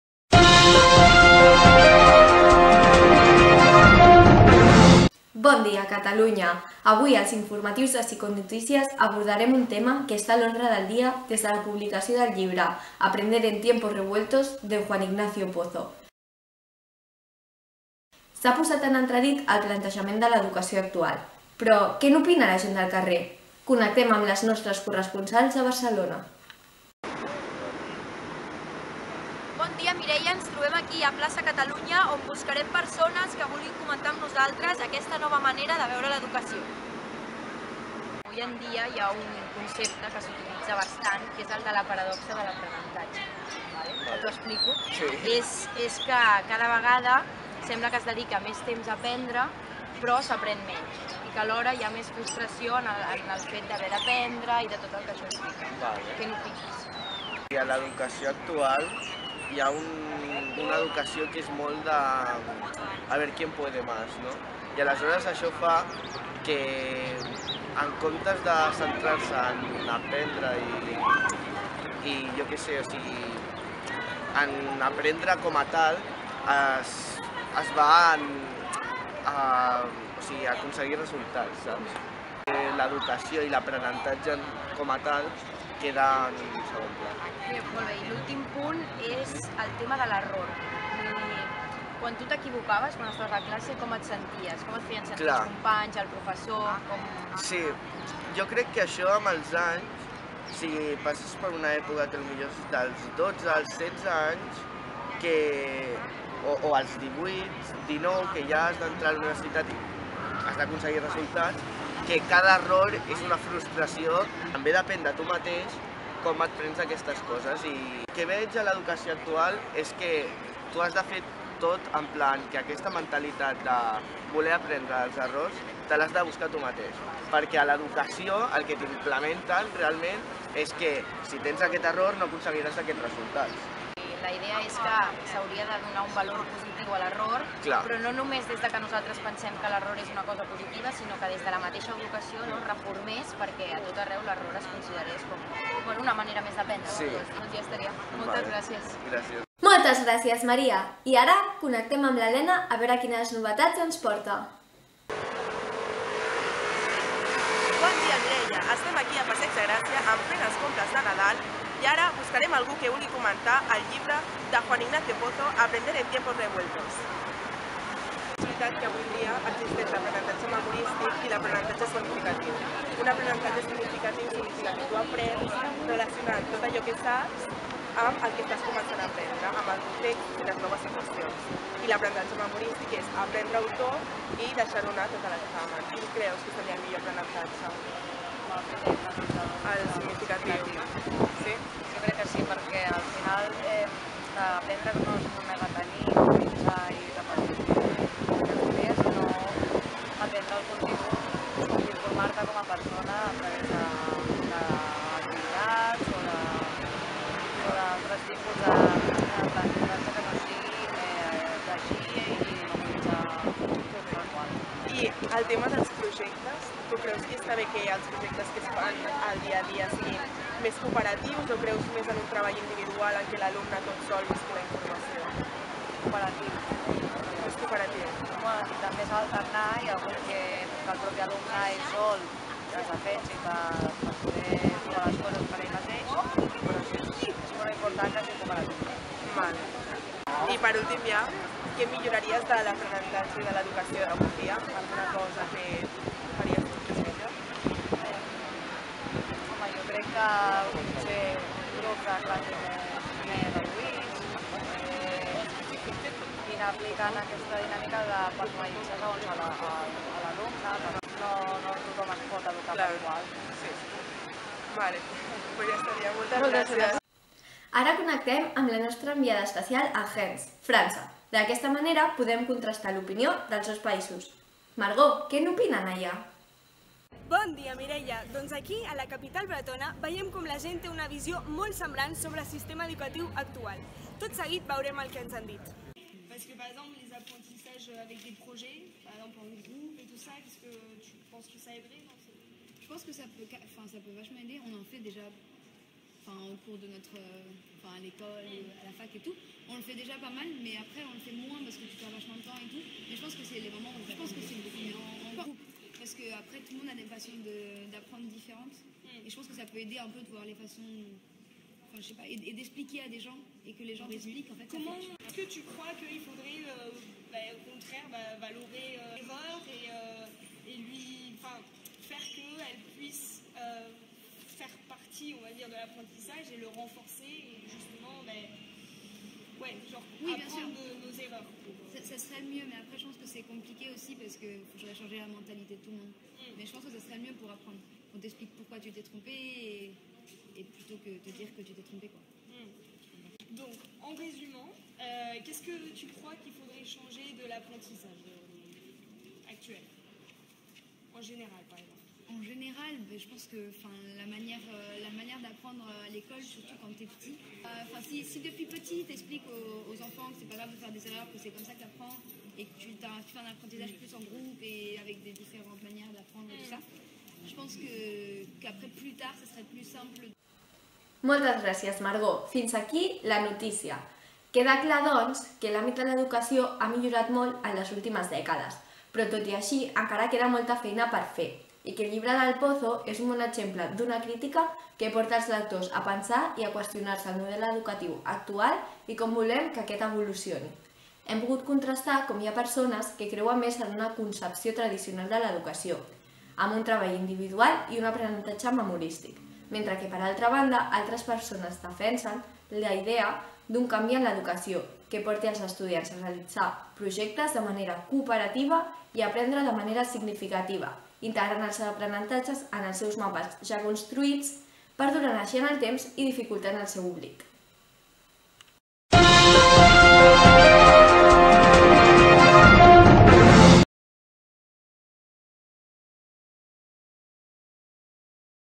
BOOK TRAILER CURT
Som estudiants de 3r de Psicologia de la Universitat de Barcelona. Aquest projecte està destinat a l'assignatura de Psicologia de l'Educació treballant el llibre "Aprender en tiempos revueltos" de Juan Ignacio Pozo.

Elena Díaz Valiente
Maria Figueras Valls
Mireia Obiols Cerdà
Margó Lorido